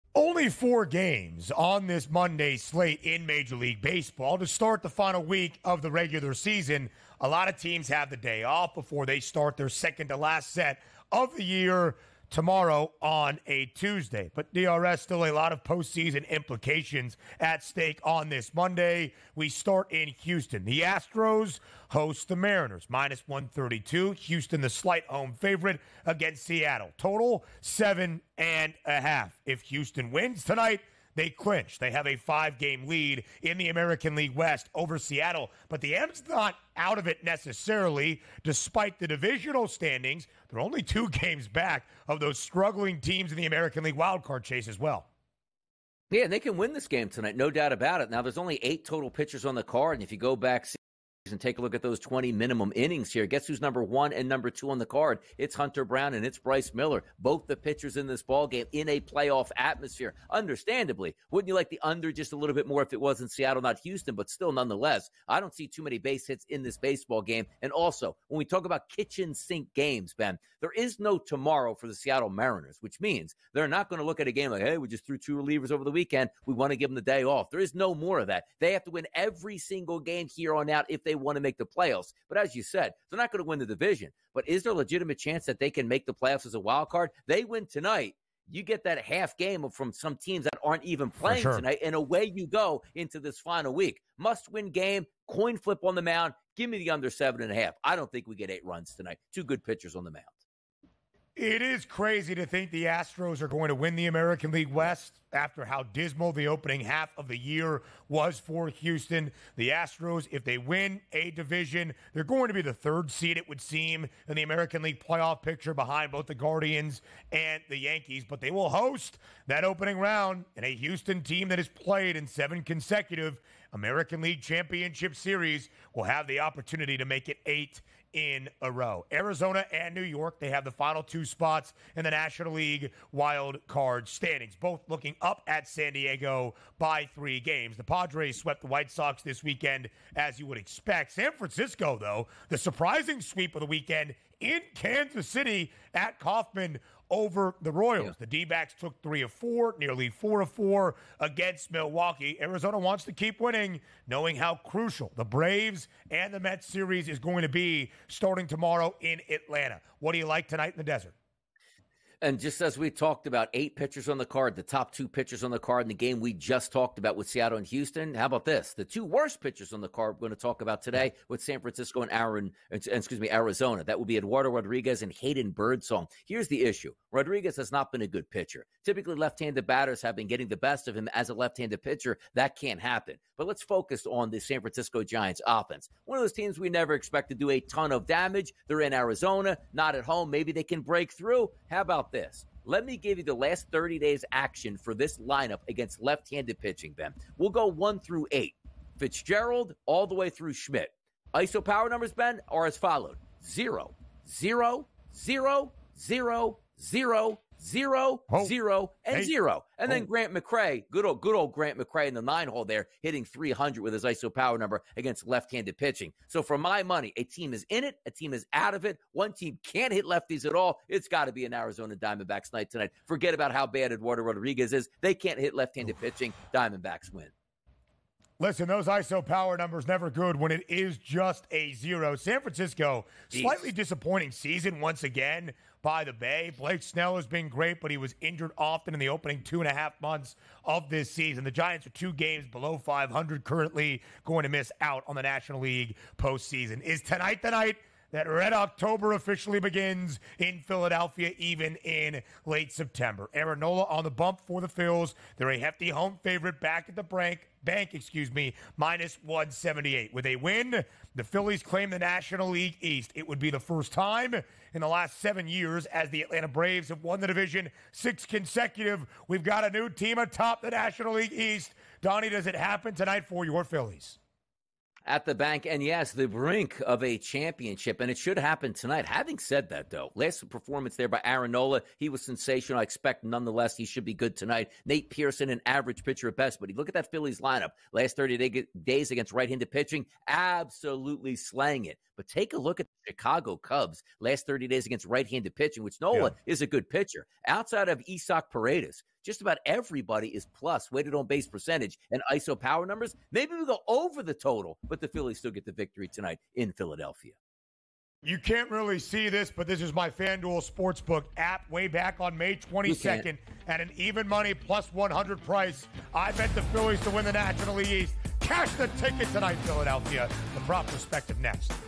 MLB Best Bets: Full Matchups and best bets for Today
This segment covers the significant Major League Baseball games on Monday, marking the final week of the regular season. Key matchups include the Houston Astros facing the Seattle Mariners, as the Astros aim to clinch the American League West, and a crucial game between the Arizona Diamondbacks and the San Francisco Giants. The analysis highlights playoff atmospheres, pitcher performance, and team dynamics. Additionally, it discusses the Philadelphia Phillies' chance to clinch the National League East. Tune in for detailed betting insights and game predictions.

00:00 MLB Monday Overview: Final Week of Regular Season
00:19 Houston Astros vs. Seattle Mariners: Playoff Implications
01:41 Arizona Diamondbacks vs. San Francisco Giants: Key Matchup
04:06 Philadelphia Phillies: National League East Showdown
05:08 Betting Insights and Predictions